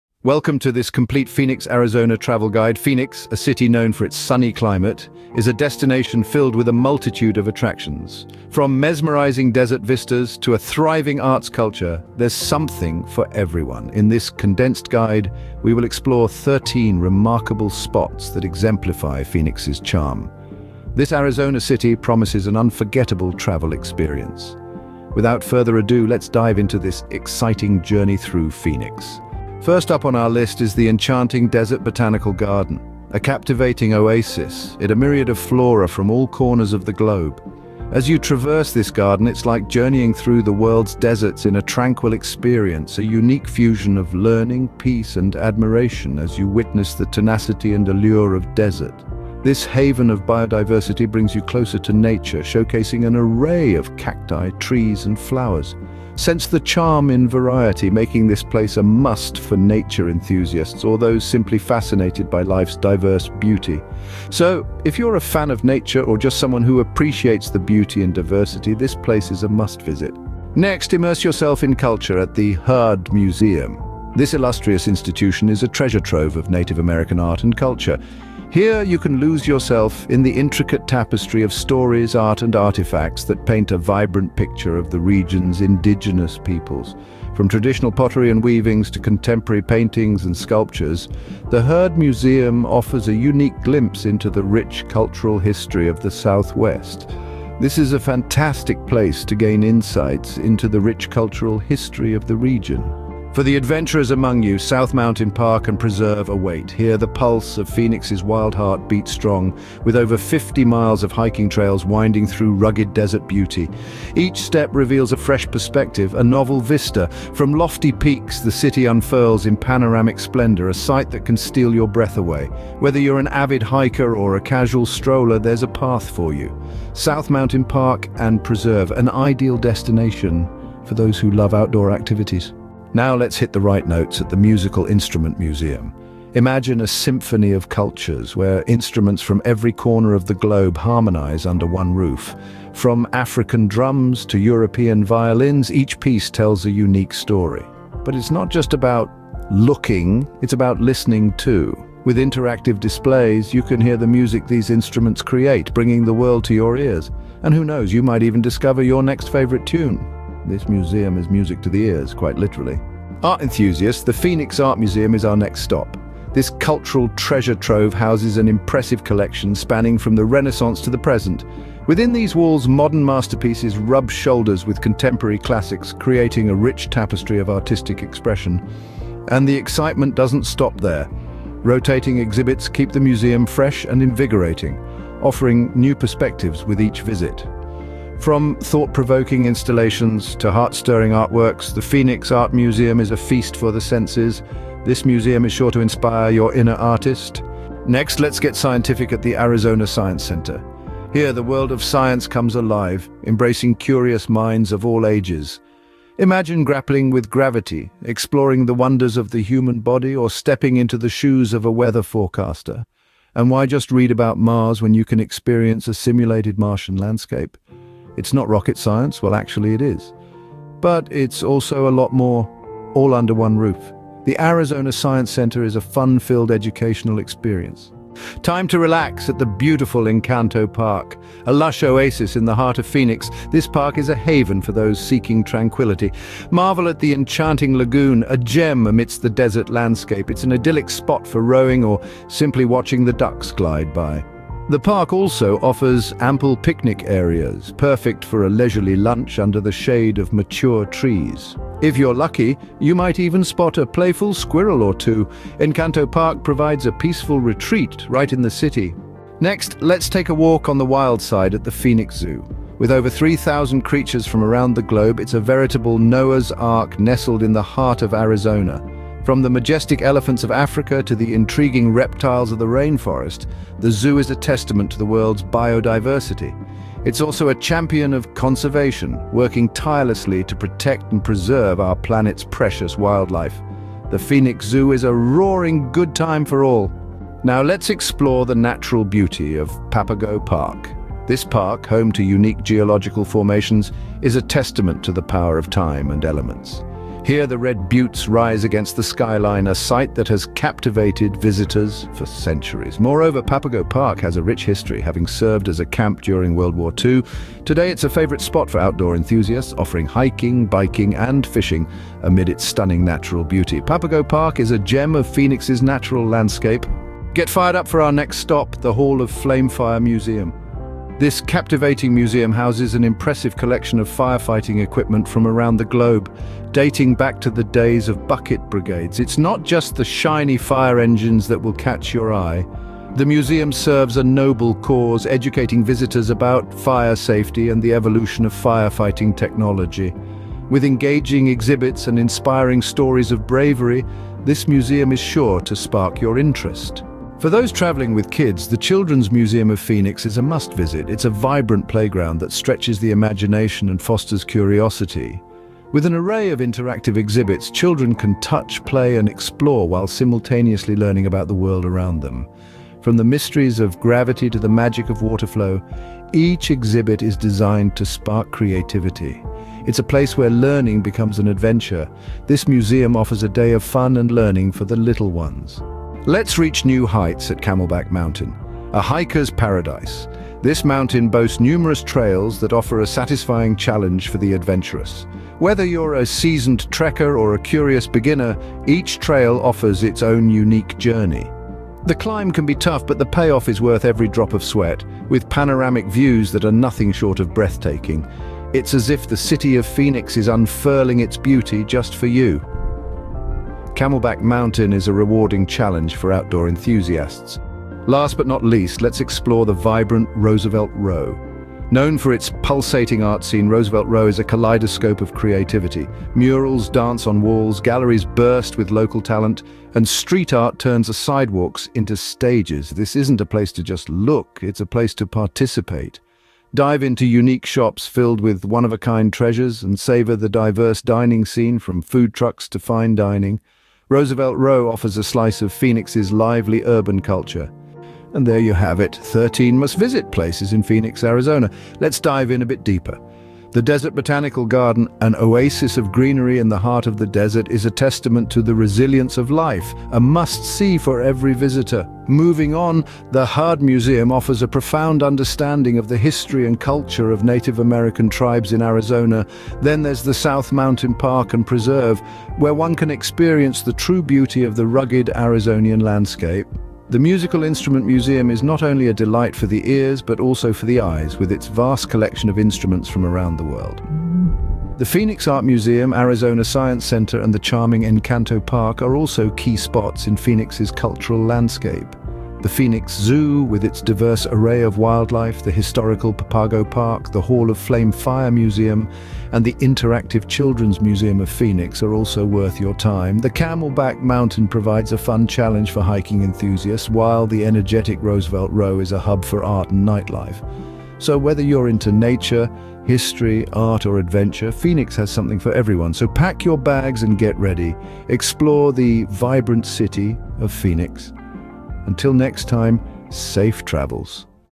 Phoenix Arizona: 13 BEST Things To Do In 2024 (Travel Guide)
Phoenix, Arizona, stands as the sun-soaked capital of the southwestern United States, renowned for its perpetually clear skies and desert landscape. It serves as a bustling metropolitan hub with a blend of urban and natural attractions, including the expansive Desert Botanical Garden and numerous hiking trails like those at Camelback Mountain. Phoenix offers a rich cultural scene with museums, performing arts venues, and a diverse culinary spectrum influenced by its Southwestern and Mexican heritage. The city is also known for its golf courses and luxury resorts, making it a popular destination for both leisure and business travelers seeking warmth and sunshine year-round.